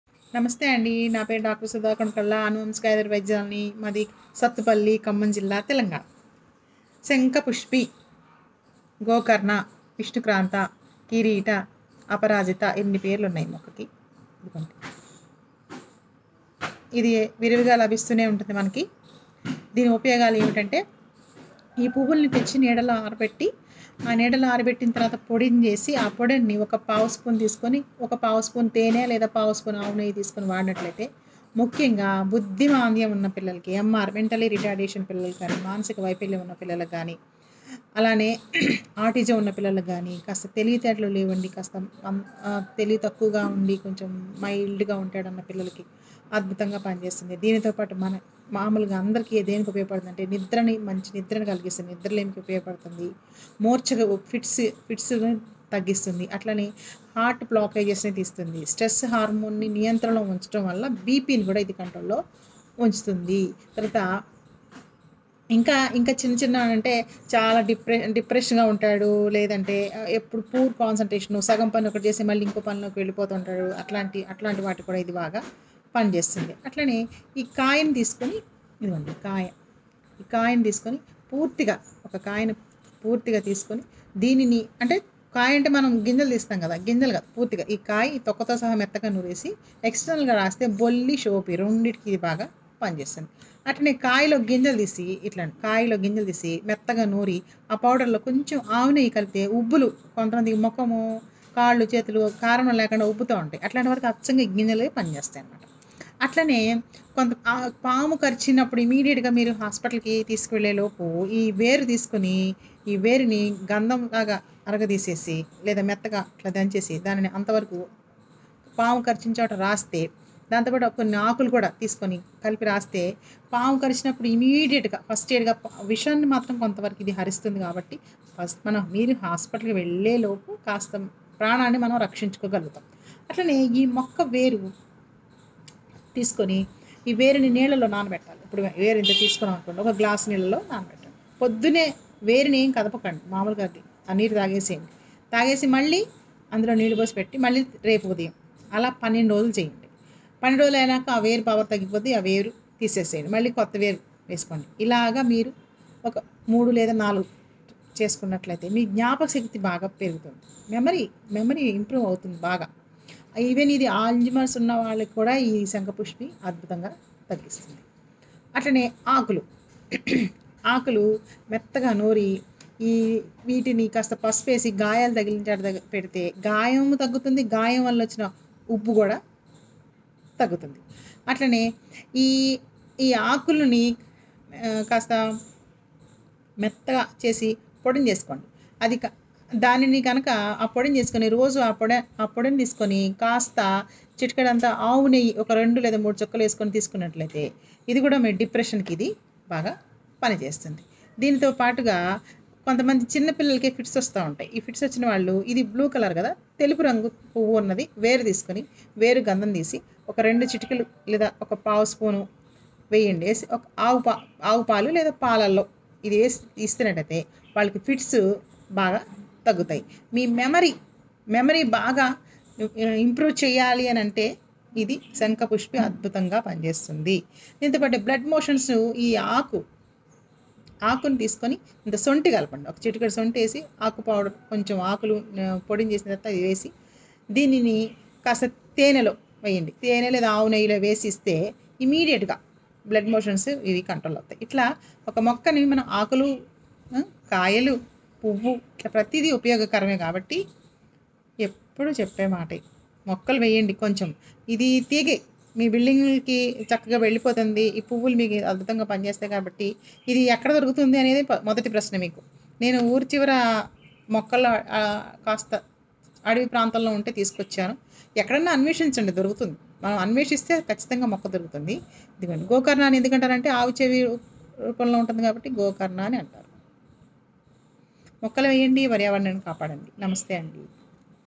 శంఖపుష్పి, అపరాజిత, విష్ణుక్రాంత, గోకర్ణ, కిరీటి ఇలా మరెన్నో పేర్లు ఉన్నాయి
శంఖపుష్పి, అపరాజిత, విష్ణుక్రాంత, గోకర్ణ, కిరీటి ఇలా మరెన్నో పేర్లు ఉన్నాయి  ఈ మొక్క ఉపయోగాలు బుద్ధిమాంద్యం ఉన్న వారి దగ్గర నుంచి ఆల్జీమర్స్ వరకు ఎన్నో వ్యాధులకు ఉపయోగపడుతుంది అన్ని విషయాలు ఈ వీడియోలో వివరించాను వీలైతే చూడండి మొక్కలు వేయండి పర్యావరణాన్ని కాపాడండి ప్రేమతో మీ డాక్టర్ సుధ కొనకళ్ళ అనువంశిక ఆయుర్వేద వైద్యురాలు సుధ కొనకళ్ళ హెర్బల్ చారిటబుల్ ట్రస్ట్ ఫౌండర్ సత్తుపల్లి ఖమ్మం జిల్లా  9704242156